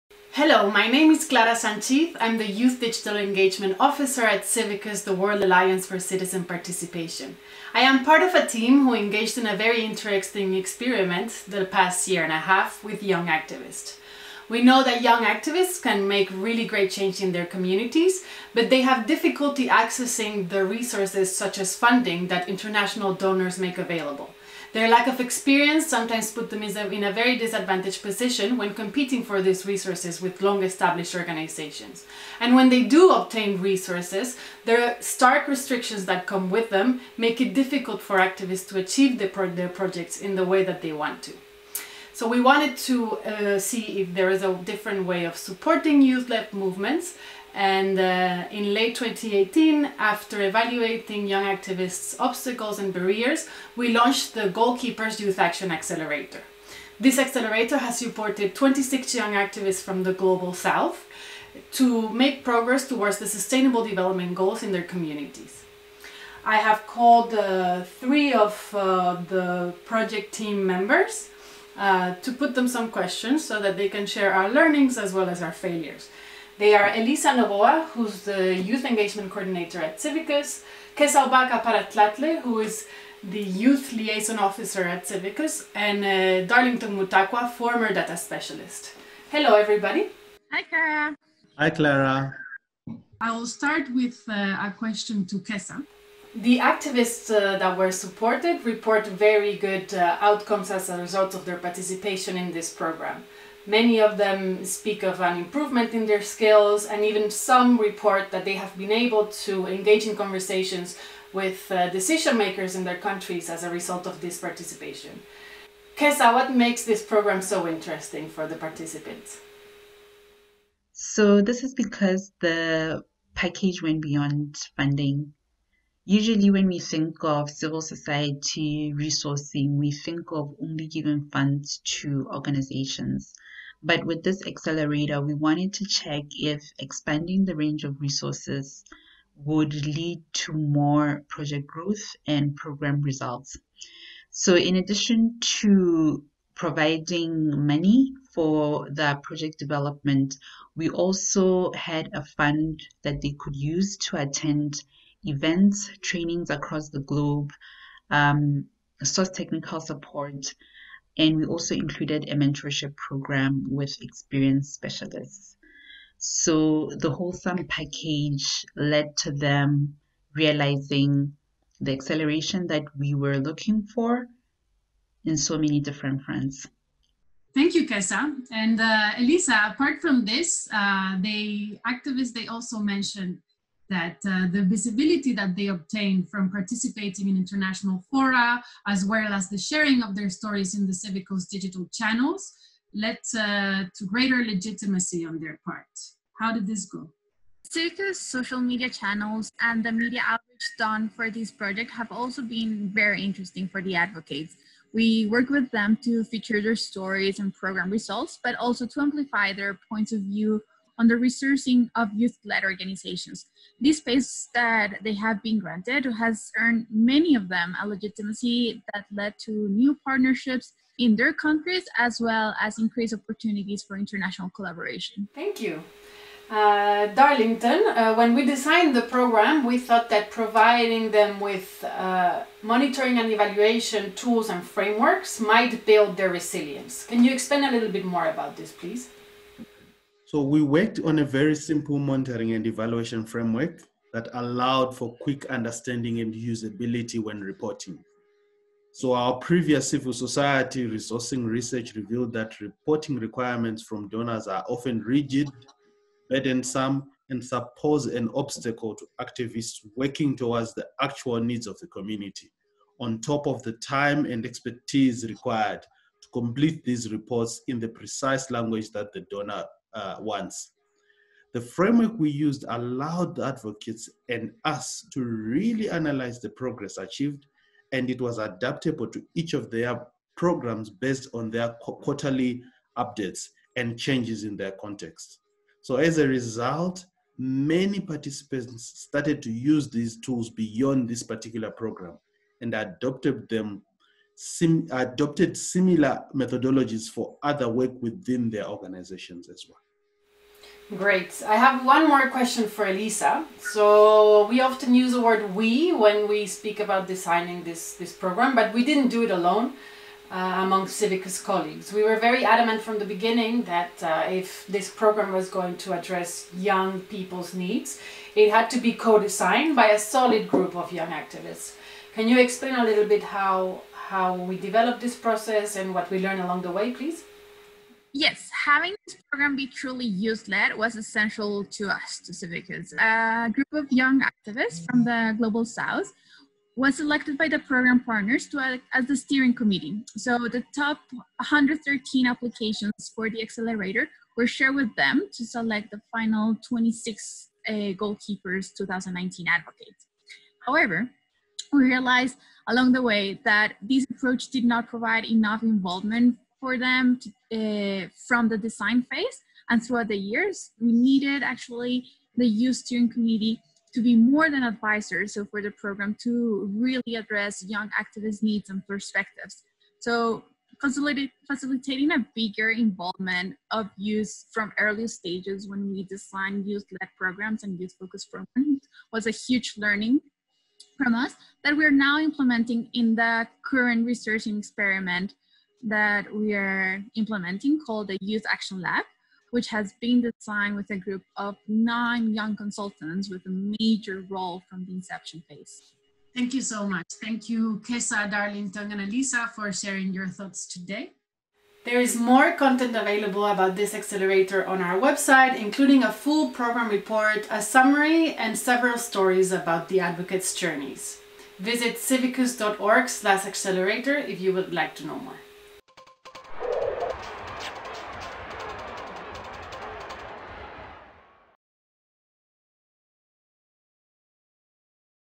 What we learned from supporting young activists to achieve the SDGs
Young people can make great change in their communities when they have resources for it, but they often find it difficult to deal with requirements that donors put in place. CIVICUS wanted to experiment with different ways of resourcing young activists to help them achieve the Sustanable Development Goals in their local communities through the Goalkeepers Youth Action Accelerator. In this video, the team that lead the experiment discusses what they learned from the program and its successes and failures.